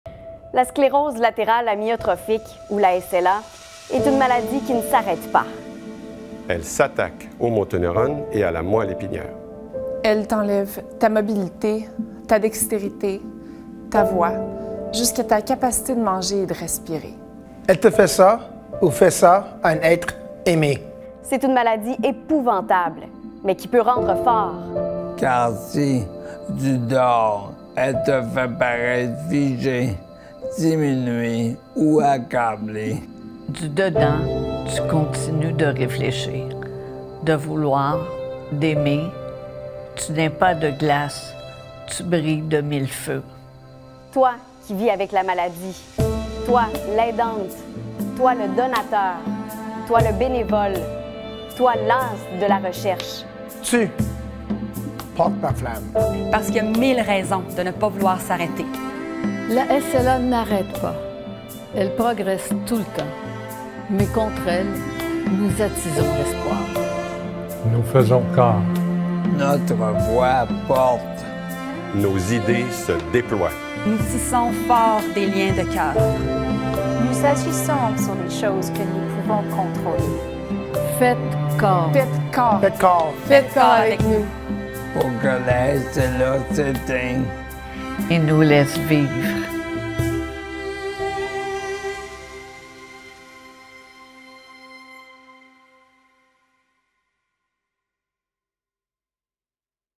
Faites corps avec nous - Manifeste SLA Québec
Ce Manifeste nous permet de nous tourner vers l'avenir, unis par une vision inspirante : que les personnes touchées par la SLA disposent des ressources, du soutien et des opportunités dont elles ont besoin pour vivre pleinement leur vie, jusqu'à ce que la SLA soit vaincue une fois pour toutes. L'objectif est d'amplifier les voix de notre communauté. Partagez-le maintenant pour que nos voix soient amplifiées ! Faites corps avec nous.

Nous tenons à remercier chaleureusement tous ceux qui ont contribué à faire de notre Manifeste une réalité. Les voix de notre communauté se font entendre à chaque mot lu par une personne vivant avec la SLA, un aidant, un chercheur, un donateur, un bénévole, un être cher ou un ami. Nos voix s'élèvent véritablement comme une seule et même voix.

Merci à :

Personnes atteintes et aidants.e.s :
Amanda Tam, Phil Lalonde, Norman MacIsaac et Marie-Christine Tremblay, Mario Hudon et Nancy Bérubé, Line Thibodout et Michel Gauthier, Carl L’Archevêque

Anciennes aidantes et bénévoles : Diane Tkalec, Judith Fetzer, Isabelle Lessard et Maria, Charline et Arielle Bourque, Susan Kennerknecht

Chercheur.eus.s : Dr. Angela Genge, Dr. Richard Robitaille

Familles/Ami.e.s proches : PJ Stock, Elias Makos, Marie-Pier Boucher, Claudine Douville

SLA Québec : Claudine Cook

Équipe de production RDS :
Louise Michaud, Réalisatrice
Charles Perreault, Directeur général RDS
Chanelle Bohémier et Sophie Bastien, maquillage
Équipe RDS : Jean-Marc Boudreault, Maxim Pitre, Julie Lacroix, Nicholas, Isabelle , Mario Pincin, Francois Perreault, Julien Boivin-Coutu, Félix Paillier, Nicolas Gaudet, Daniel Richard, Sébastien Dussault, Frédéric Villeneuve

Conception et post-production ATYPIC : Jacinthe Bussière, Arianne Cardinal, Jérémy Guiheux

Alice Films : Patrick-Joseph Duffort